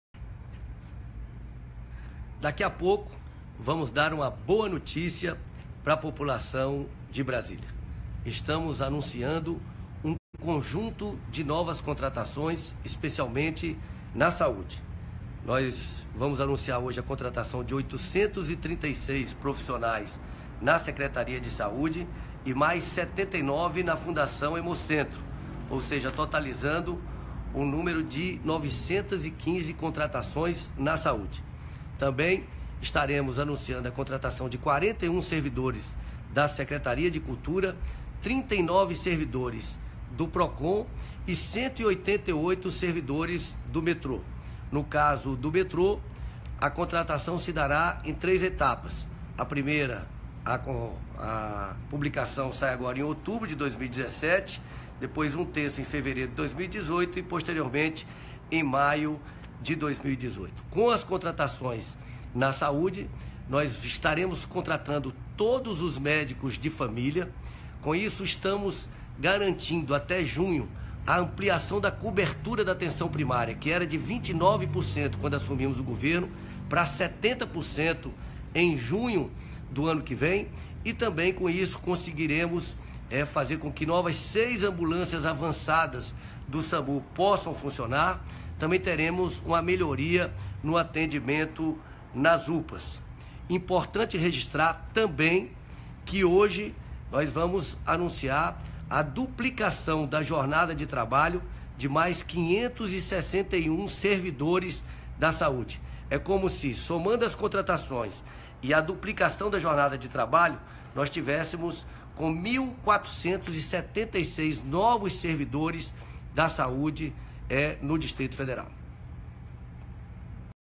Contratação de 1.183 novos servidores
Vamos contratar mais 1.183 novos servidores concursados! São 836 na Secretaria de Saúde do DF, 79 na Fundação Hemocentro de Brasília , 41 na Secretaria de Cultura do Distrito Federal, 39 no Procon-DF e 188 no Metrô Brasília. Além disso, vejam que bacana: foi autorizada a ampliação da jornada de 561 servidores da Saúde, o que contribui muito para a melhoria do atendimento.
Leia mais aqui: goo.gl/JMvzJC